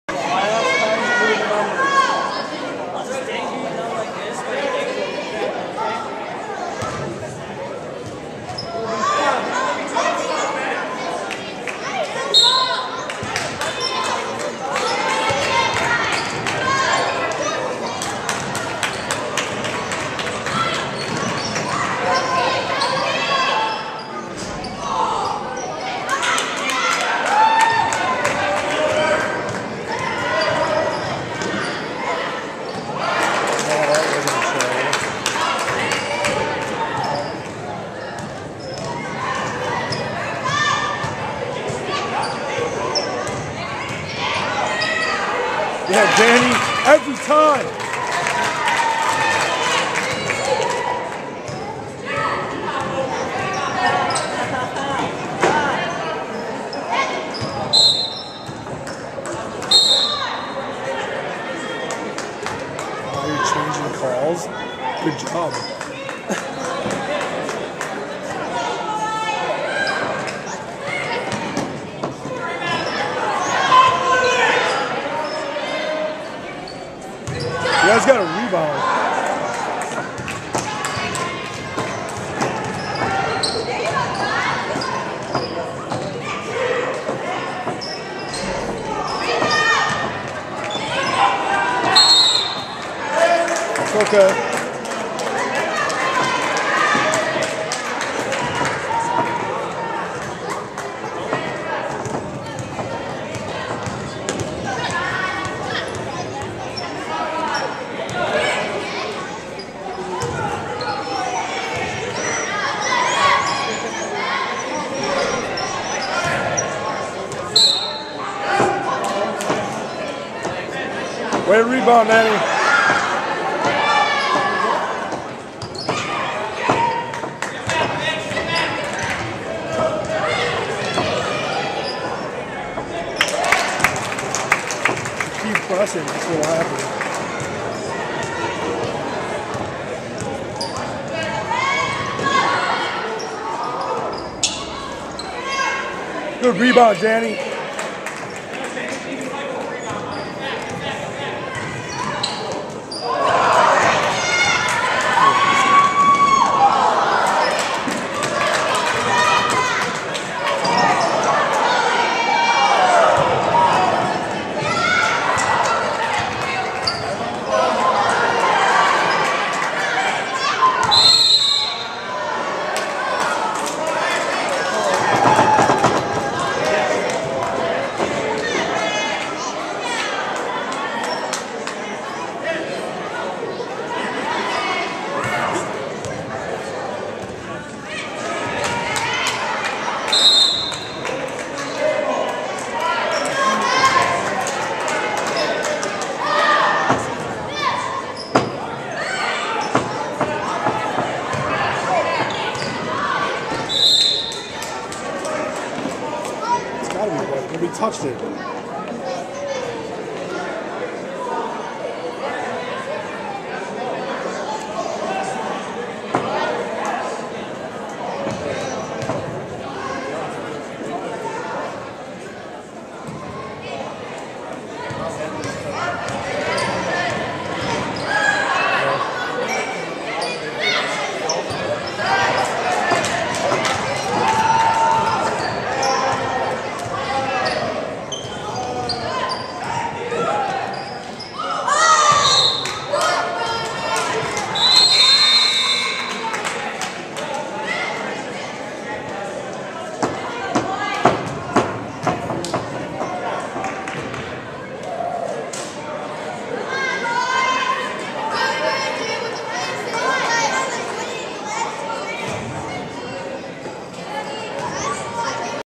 4.1Q - Brookfield WI - 6th Grade Boys 2009-2010 Finals -
Brookfield WI - 6th Grade Boys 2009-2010 Basketball Finals
Bulldogs v Phoenix